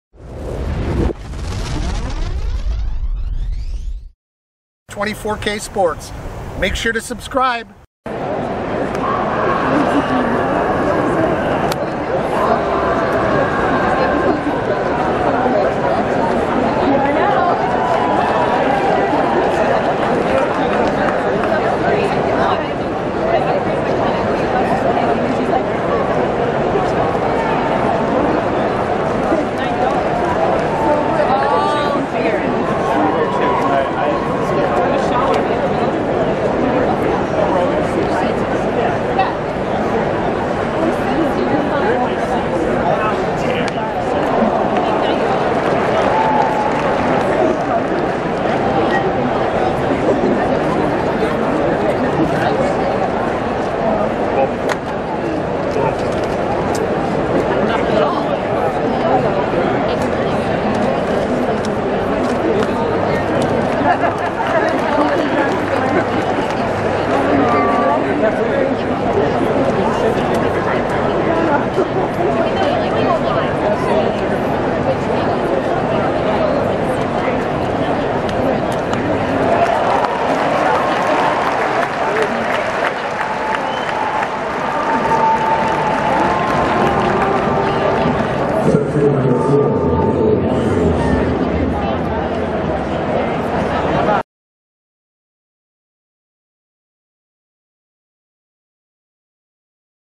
Josh Naylor's First MLB at Bat
Josh Naylor's First MLB at Bat - May 24, 2019 Rogers Centre Toronto as a member of the San Diego Padres

Canadian gets his first MLB at bat on Canadian soil

Interested in Sports Memorabilia?  Have a look at our eBay Store